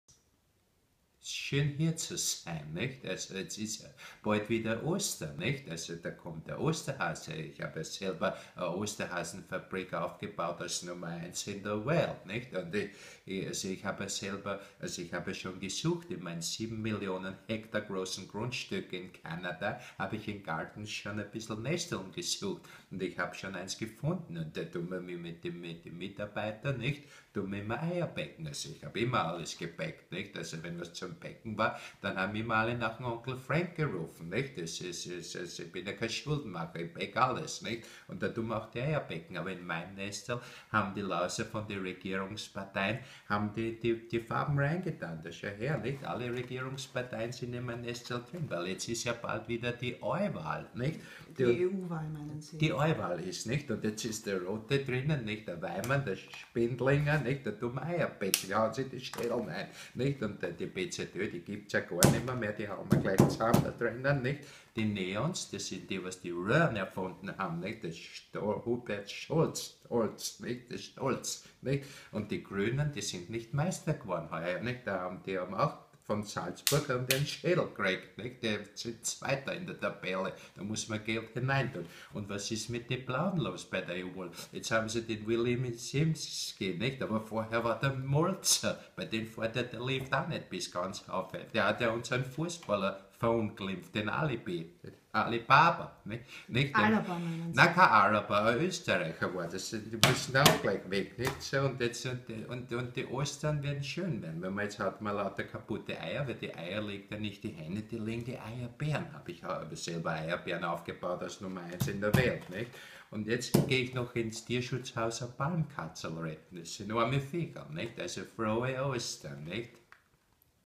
Alex Kristan und die Frank Stronach Parodie
Frank Stronach wünscht frohe Ostern